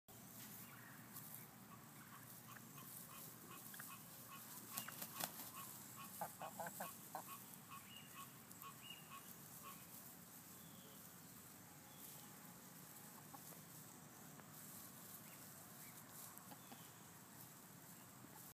Chickens Coming Back To Coop
The chickens are making their way back after a day of ranging.